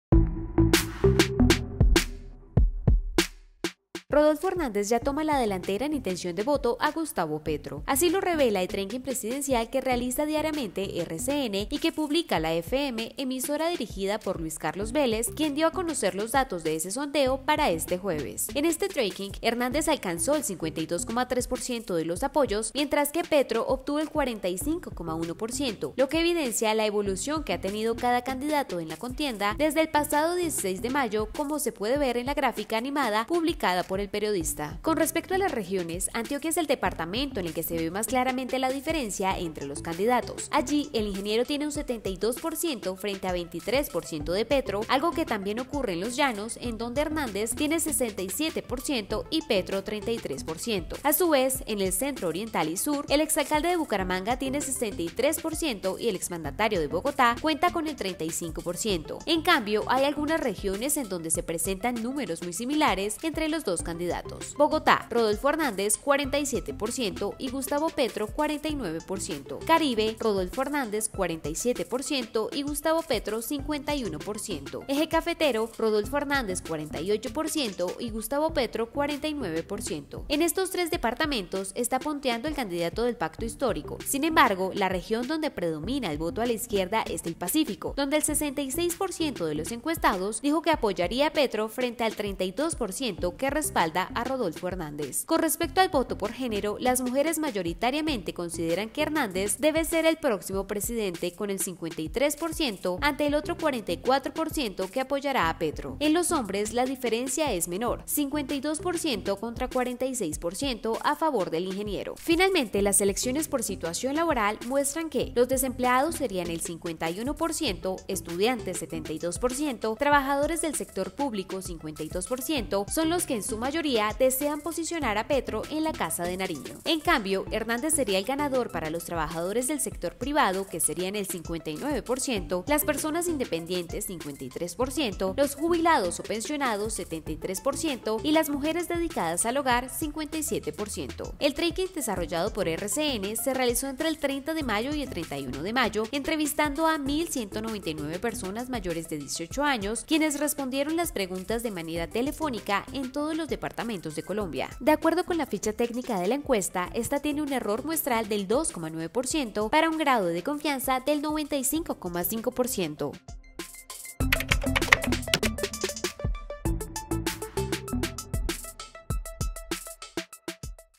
Rodolfo Hernández aventaja a Gustavo Petro en encuesta de RCN.
El tracking se desarrolló entre el 30 y 31 de mayo y, de manera telefónica, consultó la opinión de más de 1.000 colombianos sobre la segunda vuelta presidencial.